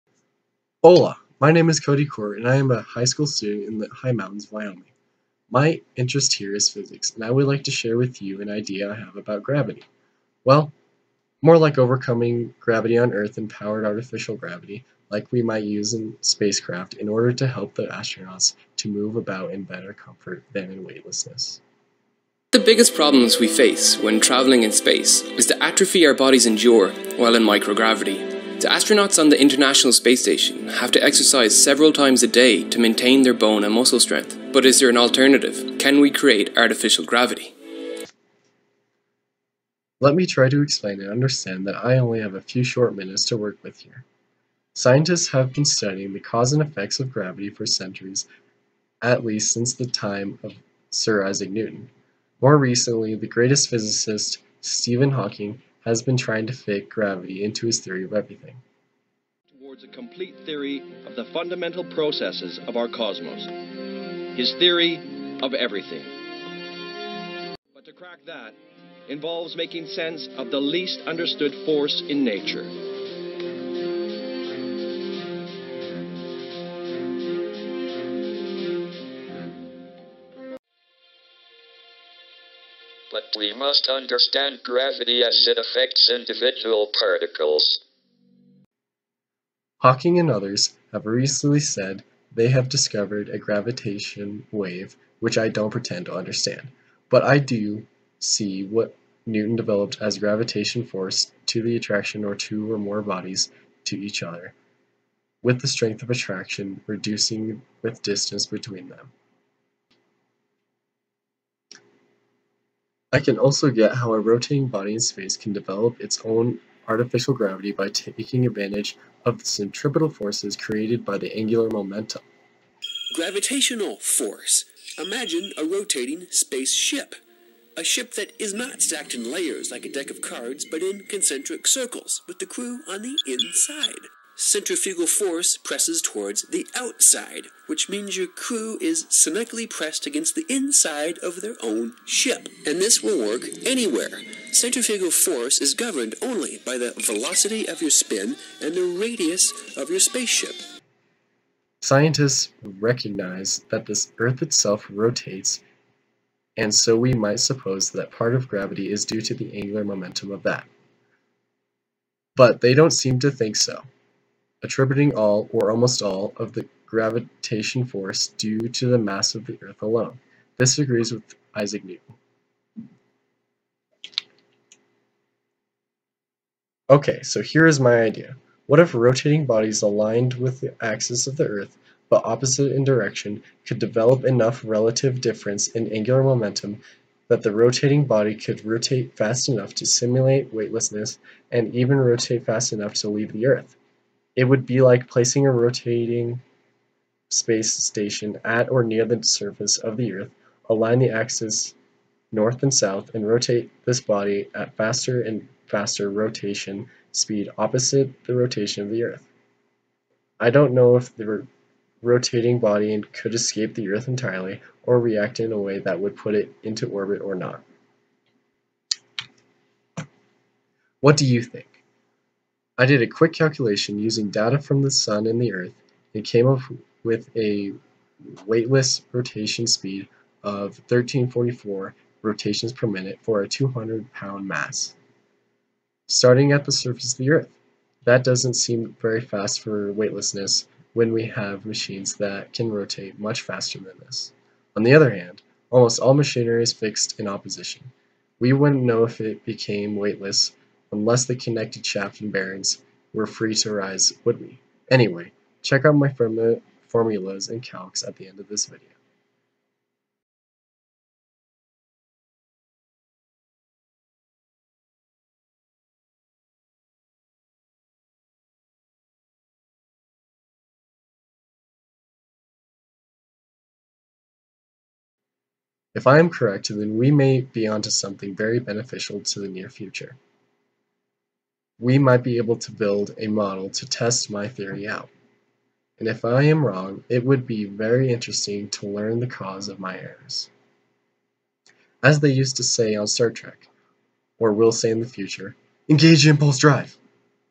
Cody Cor's AntiGravity Theory
This is a high school student's formula and explanation for his theory of antigravity, inspired by the work and studies of Sir Isaac Newton and Stephen Hawking.  His hypothesis is that a rotating body, similar to a revolving space station, could be aligned at or near the earth's surface, rotated opposite to the earth, and apply power to develop a counterforce to natural gravity.  His formulae and reasoning are subject to further development and possible modeling in a laboratory environment.  Possible breakthrough for the benefit of society at large and into the future.